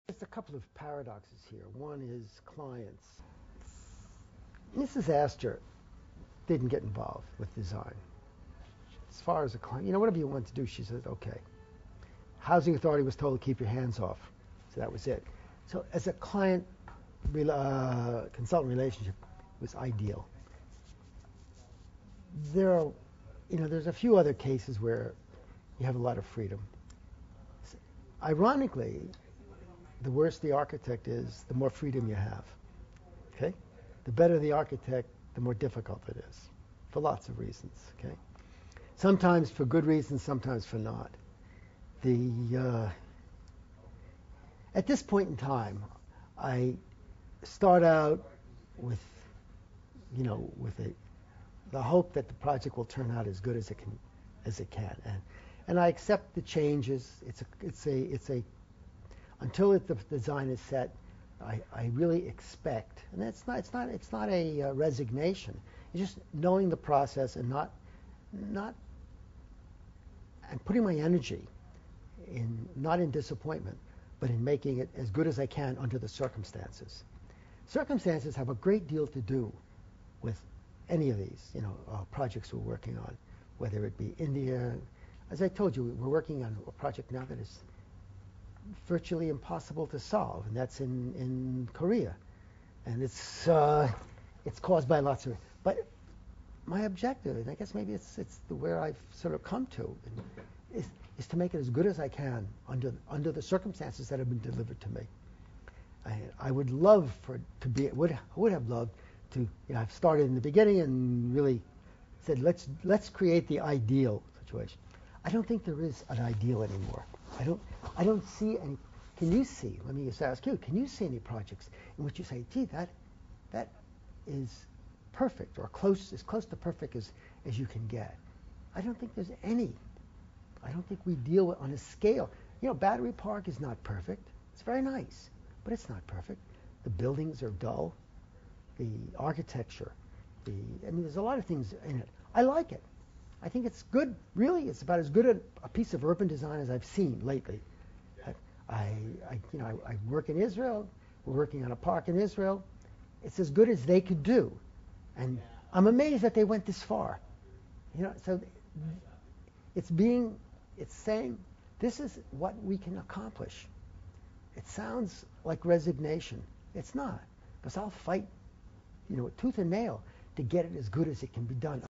M. Paul Friedberg on Design: Getting Good Projects Built
Clients and circumstances influence how a project is realized.
Interviewed by Charles A. Birnbaum, October 2006.